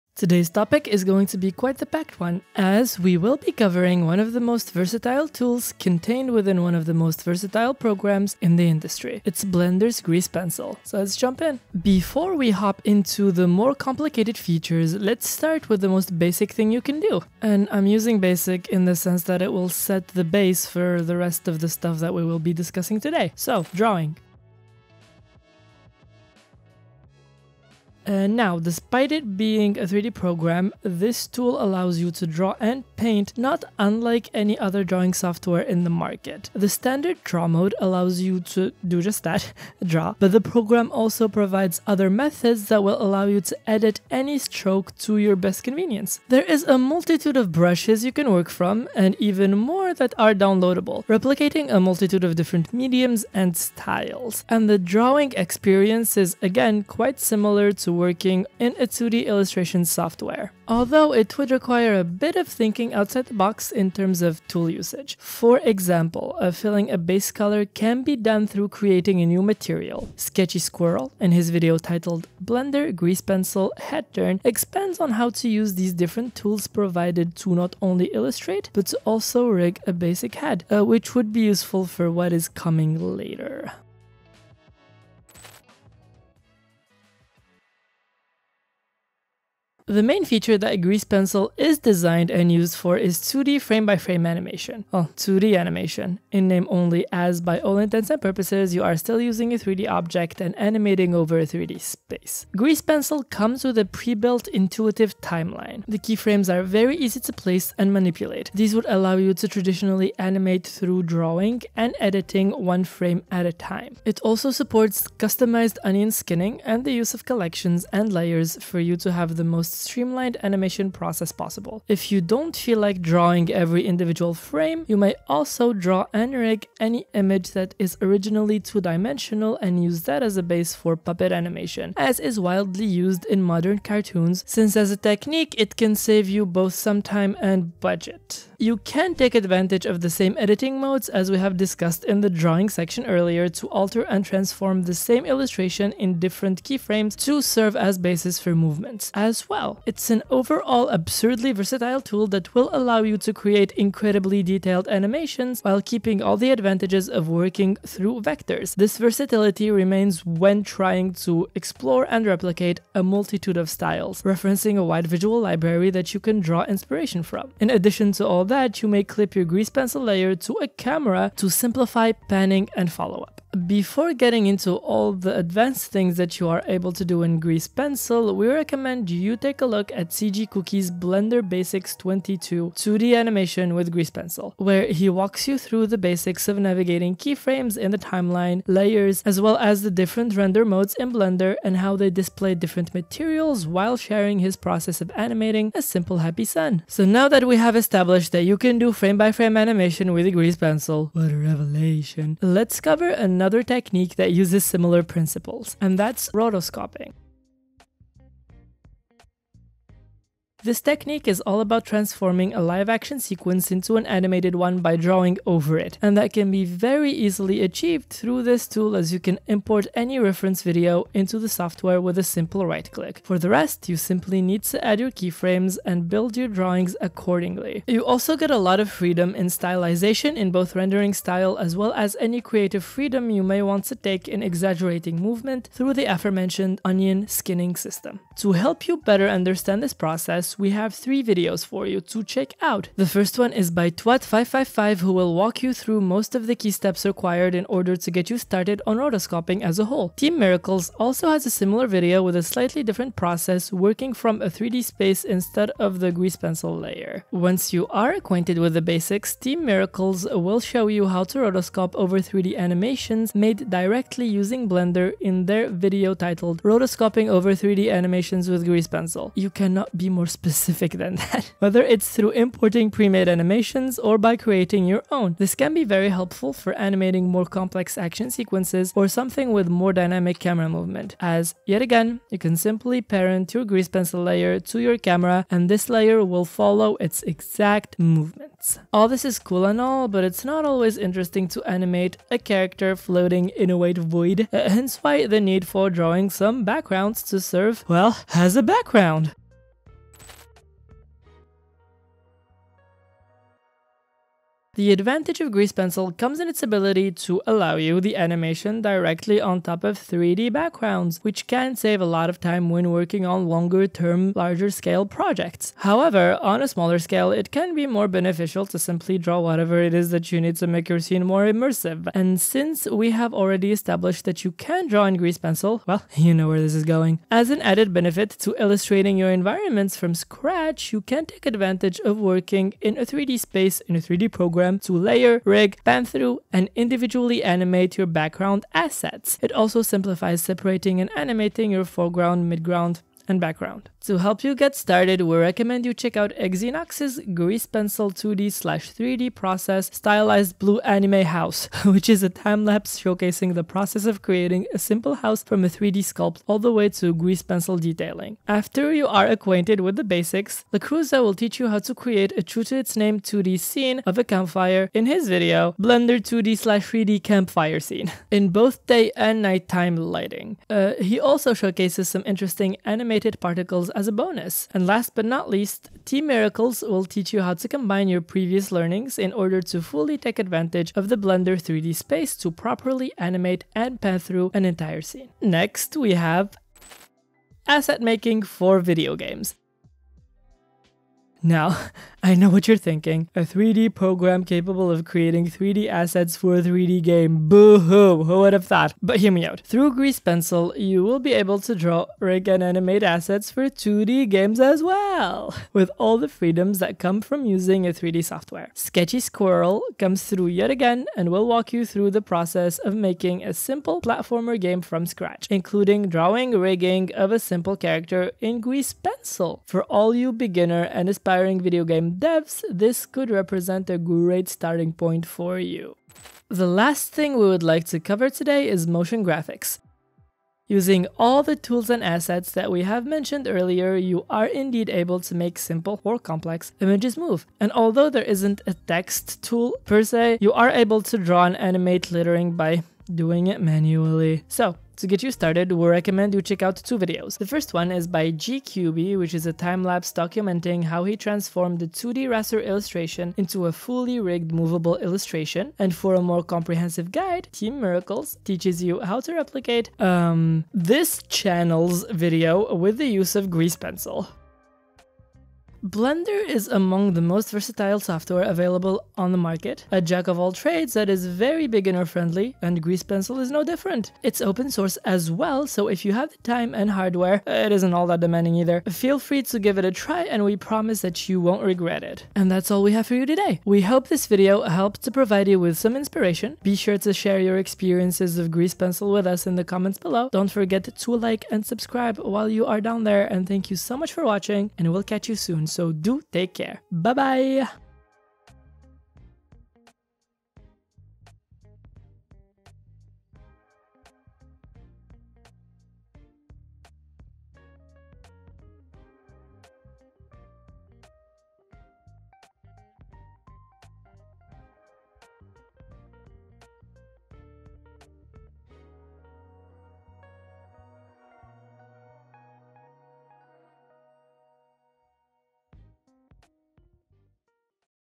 The Best Things You Can Do With Grease Pencil
In today's video, we will be covering one of the most versatile tools contained within one of the most versatile programs in the industry, it’s Blender’s Grease Pencil. Let’s jump right in!

00:00 Intro
00:24 Drawing
01:26 Animation
3:28 Rotoscoping
05:02 Environments
06:28 Video games
07:06 Motion graphics